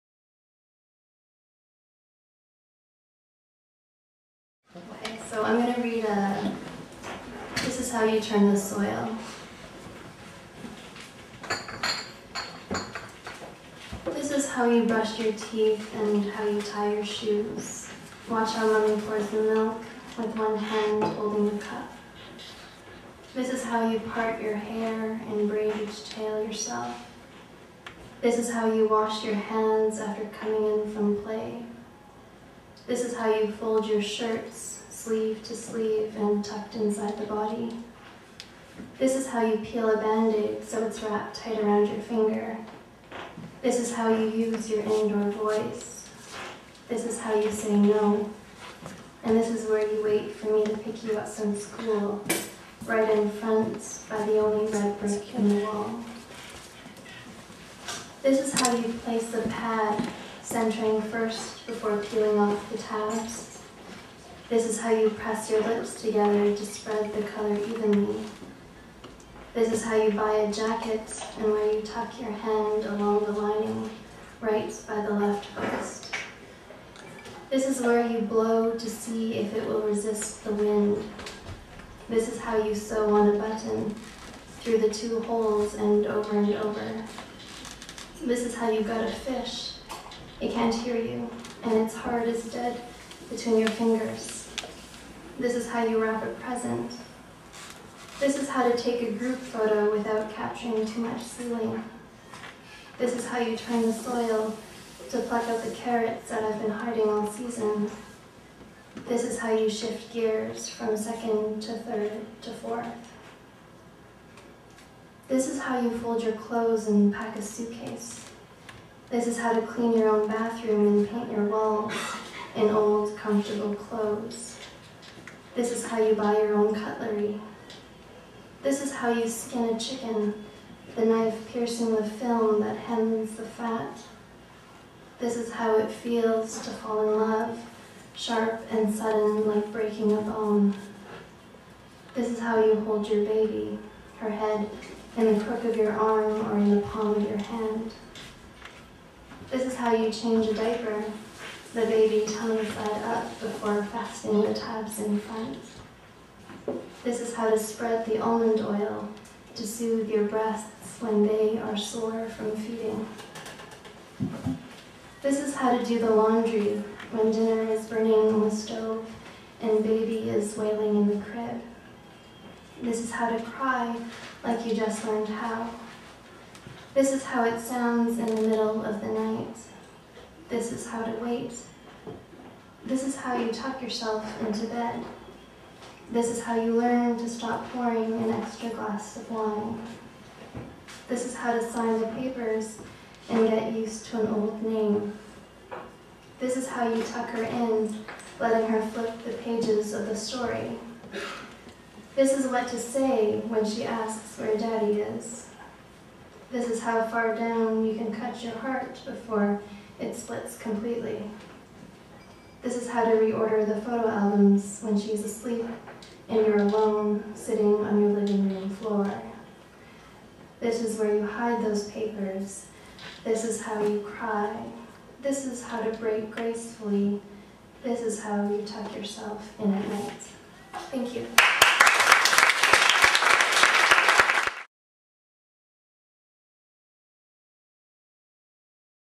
Gillian Sze reads from The Anatomy of Clay.m4v
Gillian Sze reads from her second poetry collection, The Anatomy of Clay (ECW Press, 2011).